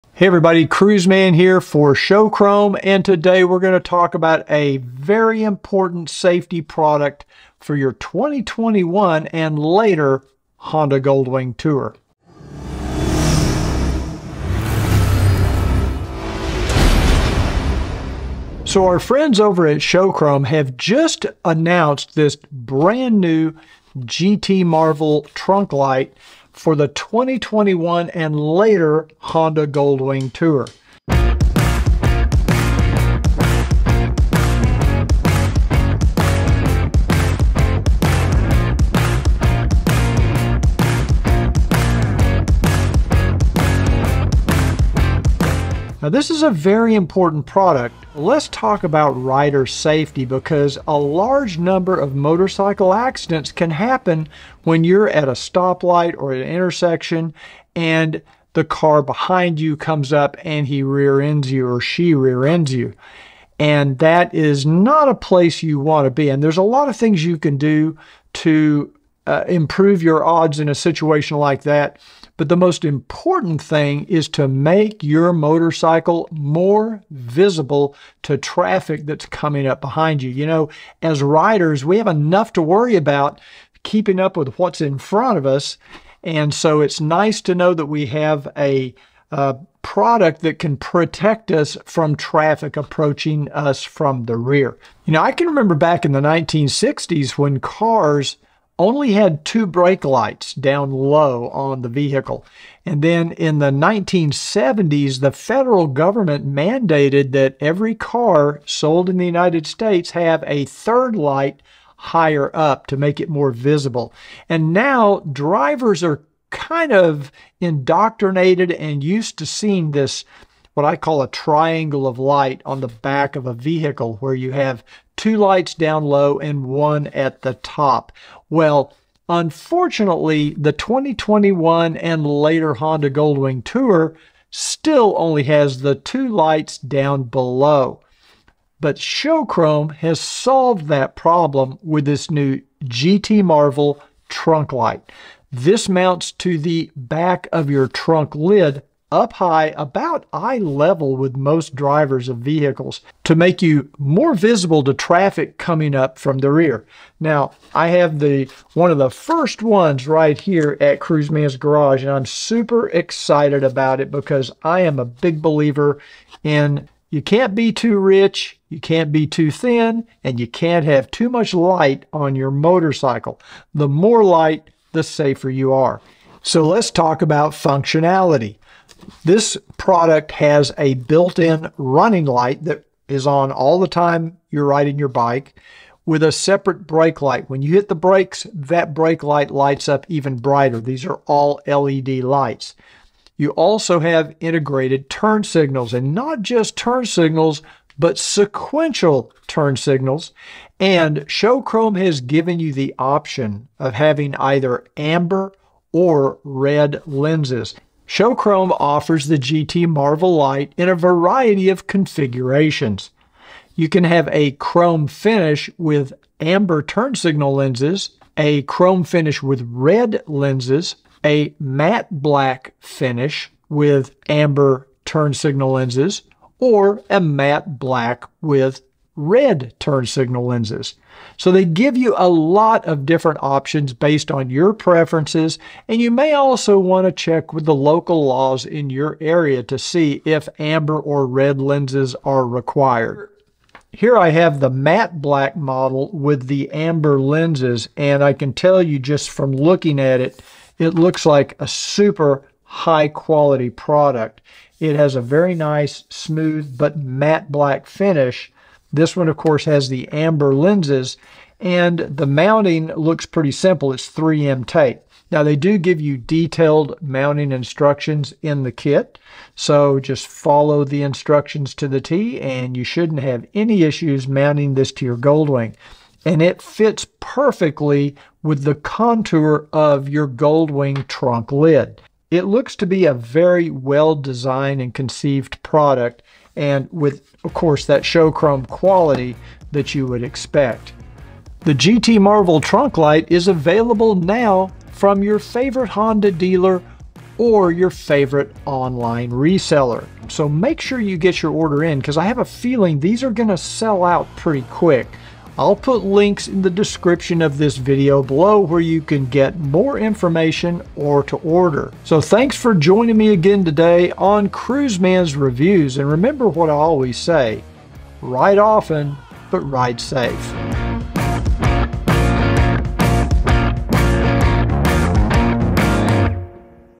GT Marvel Trunk Light
This running, brake, and sequential turn signal masterpiece offers options for sleek black or brilliant chrome finishes with all-red brake and turn lights, or traditional red brake lights with amber turn signals. The light measures 21" wide x 1-7/8" tall. Includes the option to wire as a sequential or standard flashing turn signal. Increase your safety and boost your style on the road!